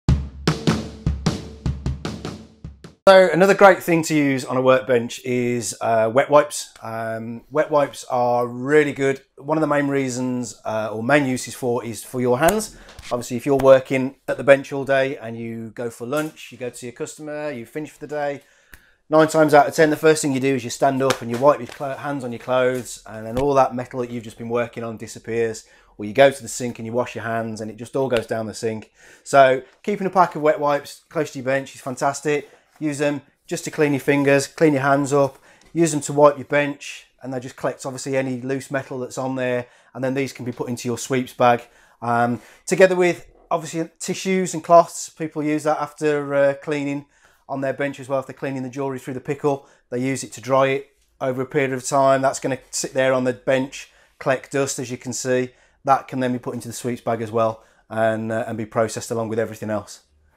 Wet Wipes in a jewellery workshop
Wipe your hands with wet wipes after polishing or working at the bench. Regularly wipe down all the surfaces in your workshop and throw the used wipes into your sweep bag.